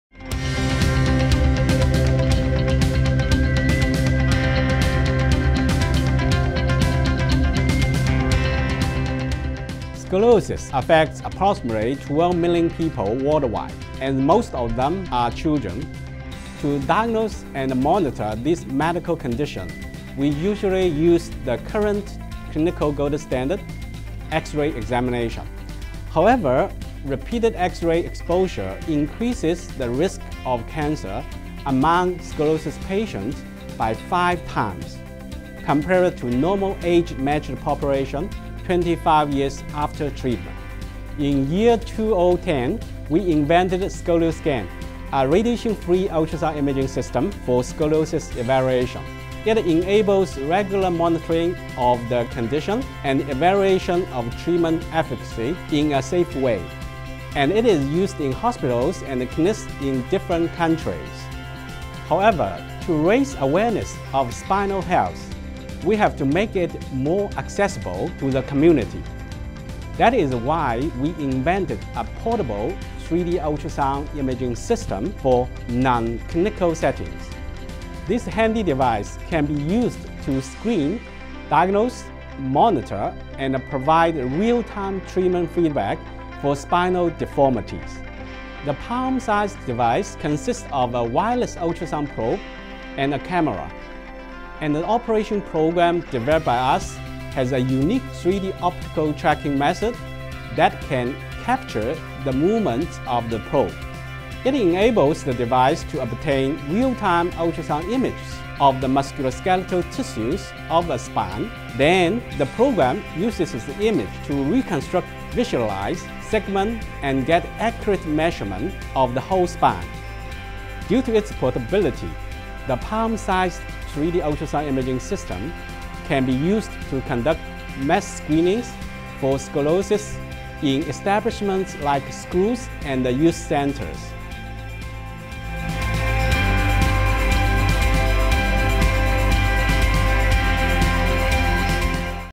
Palm-sized 3D Ultrasound Imaging System for Radiation-free Scoliosis Assessment
PolyU has developed a portable 3D ultrasound imaging system for radiation-free assessment of scoliosis to diagnose scoliosis in early stage, to provide monitoring of treatment outcome, and give real-time feedbacks during treatment.